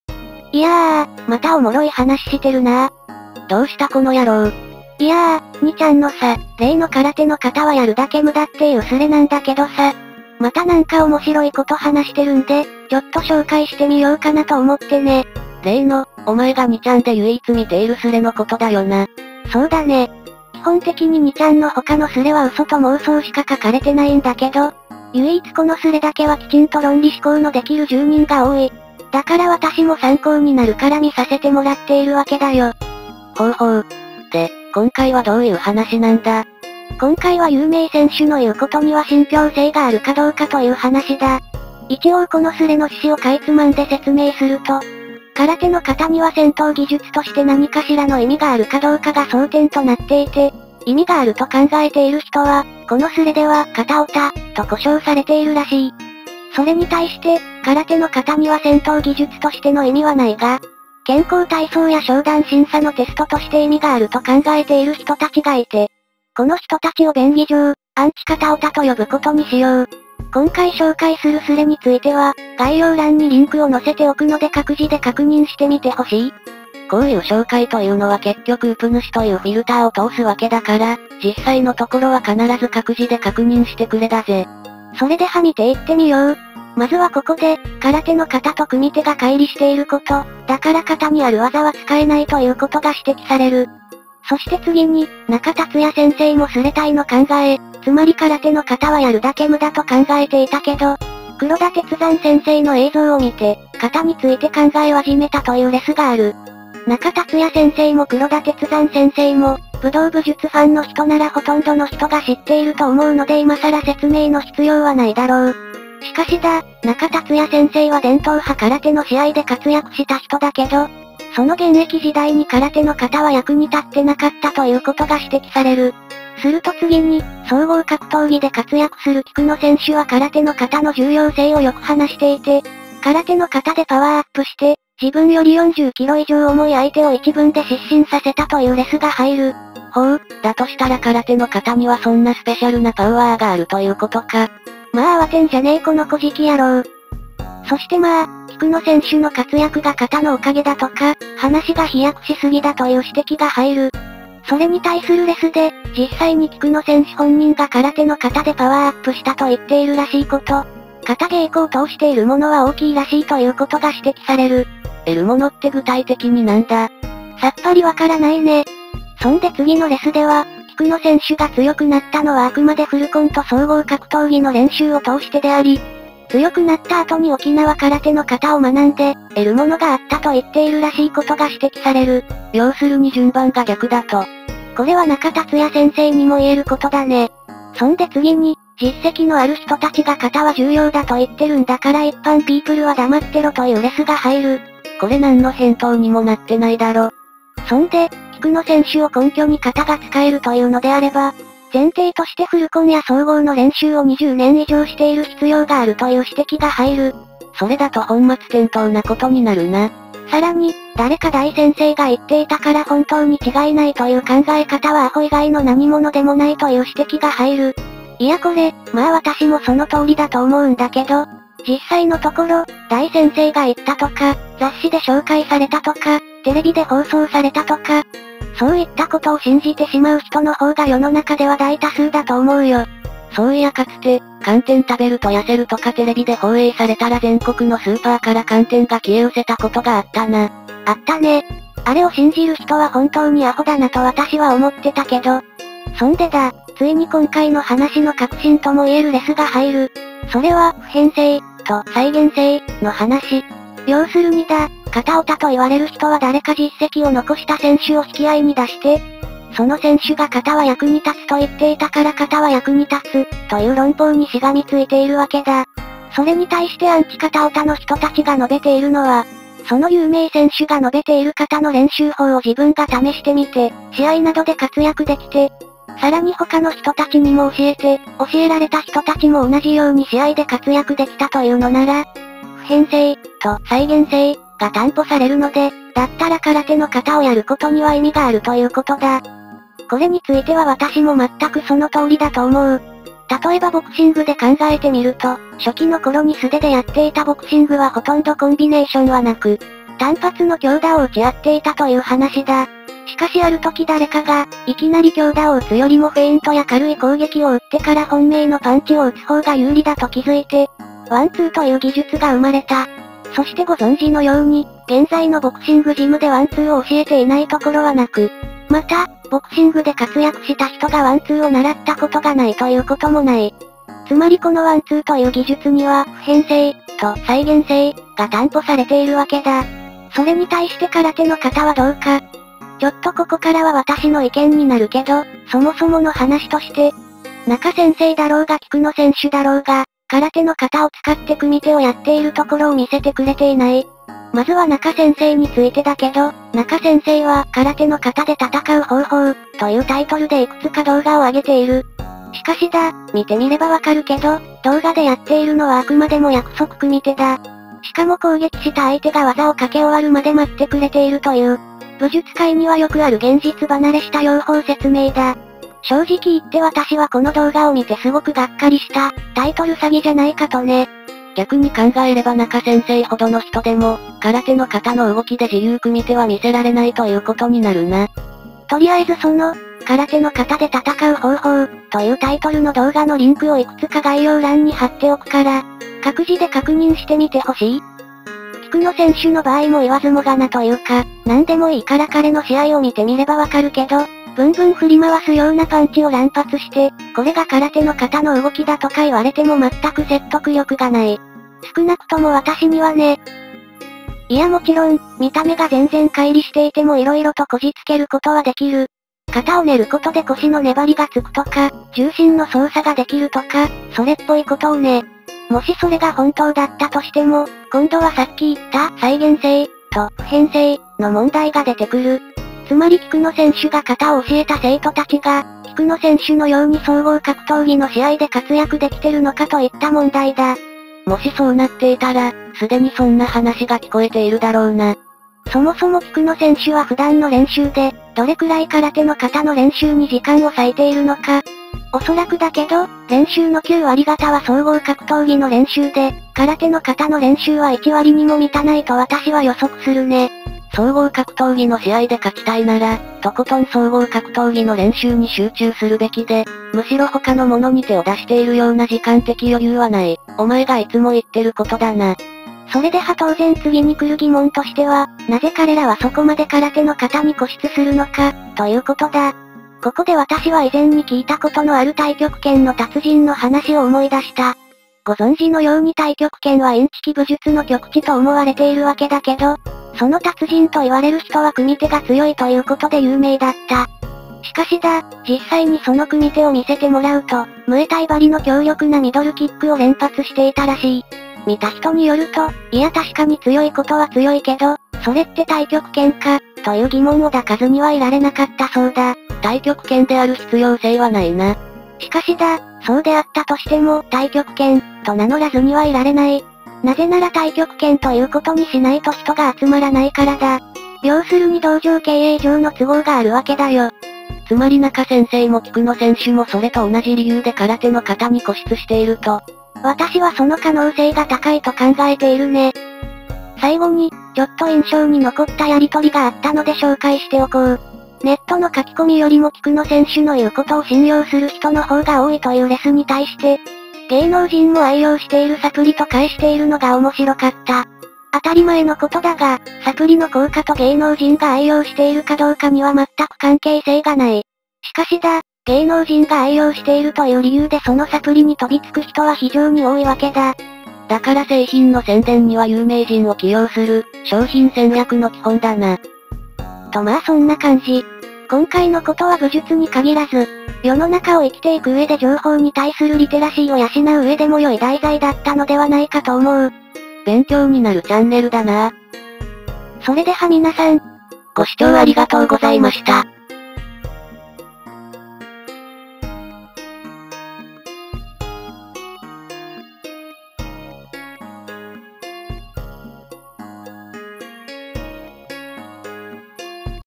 有名選手の言う事ならば本当なのか？：2chスレ「空手の型はやるだけ無駄という現実」より
AquesTalk2ライセンスID
AQTK2WINUSR19122029

2chスレ「空手の型はやるだけ無駄という現実」

こちらのちゃんねるもよろしくお願いします(^▽^)/
twitterもやってます。
ゆっくり武術解説ちゃんねる